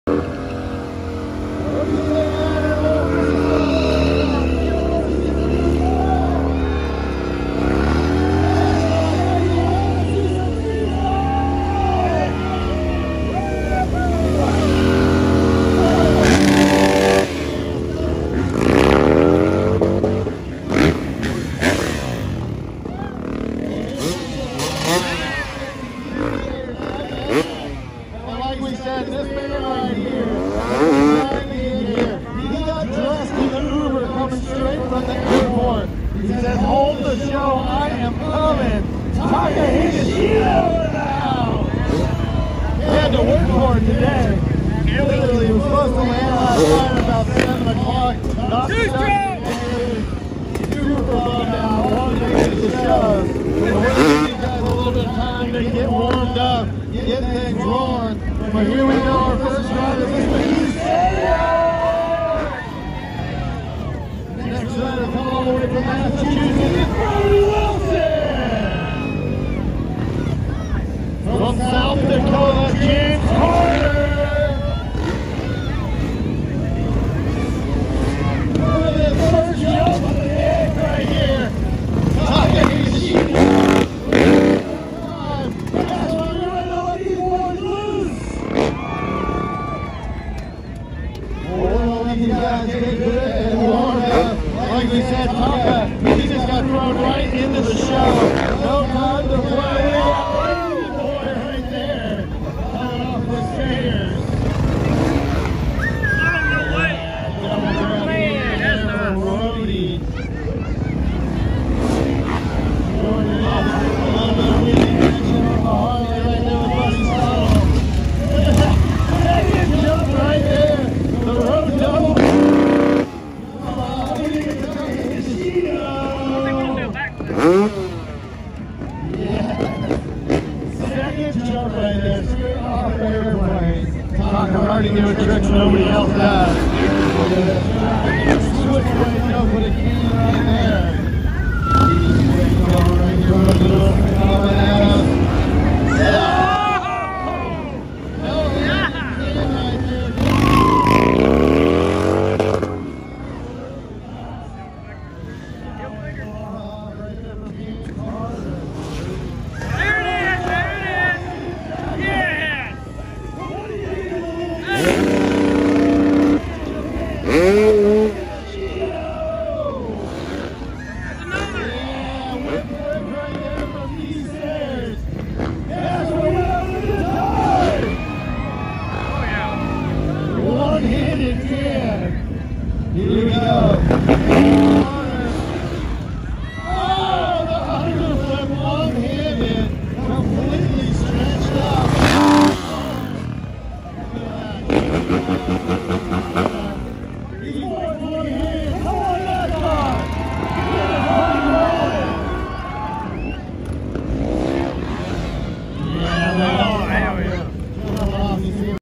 Freestyle Motocross 2021
This event was at nascars inaugural alley 400 at Nashville speedway. Monster energy riders put on a pre race show that was insane.
And to the subscribers that watch be the first comment SubEnergy1 and u will win a free $20 gift card